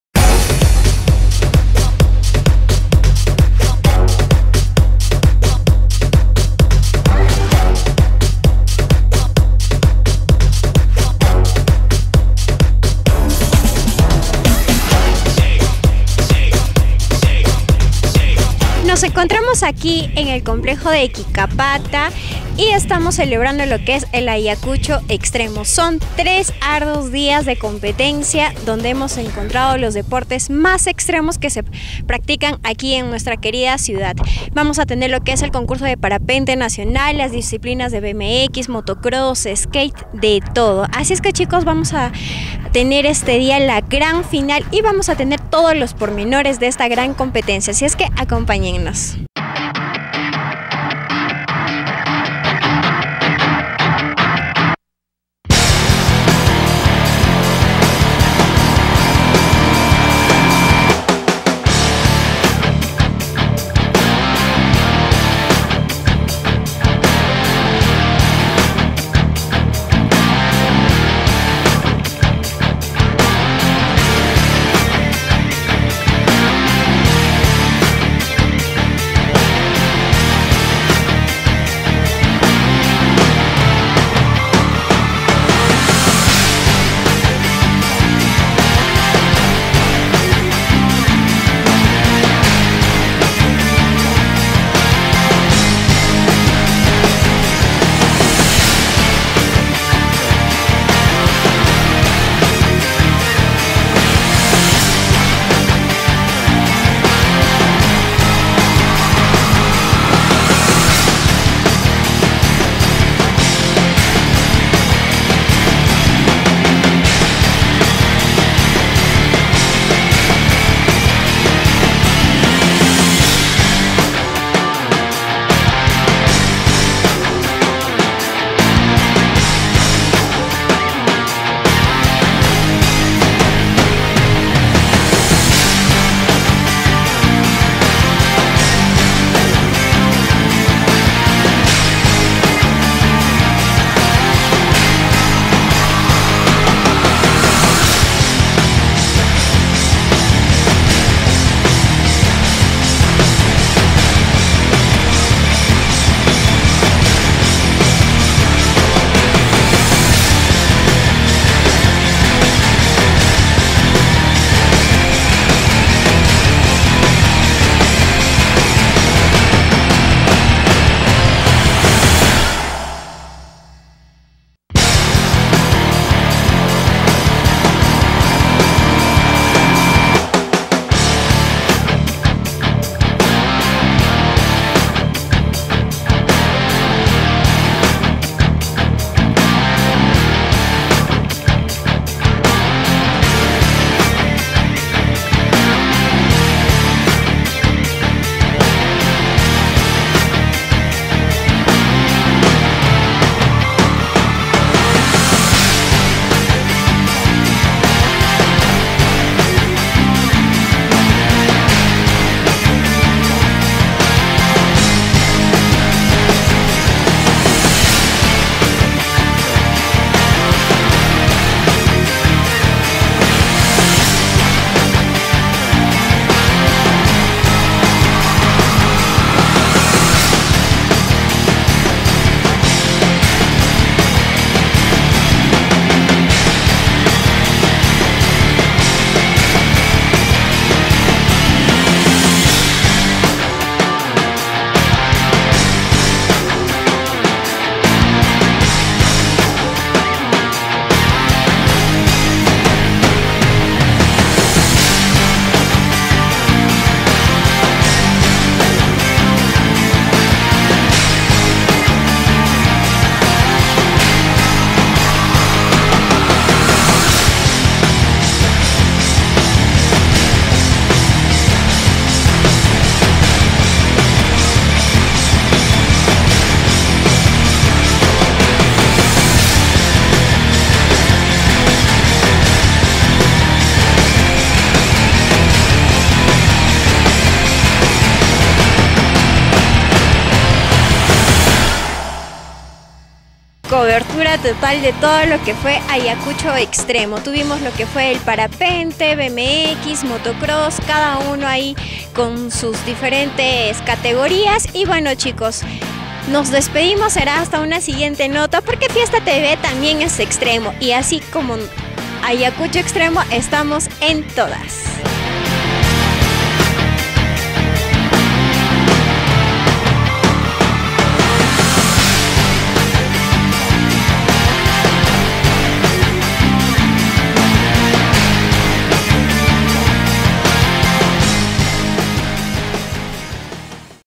BMX en Ayacucho Xtremo - FIESTATV AYACUCHO
Aquí las imágenes de BMX en Ayacucho Xtremo.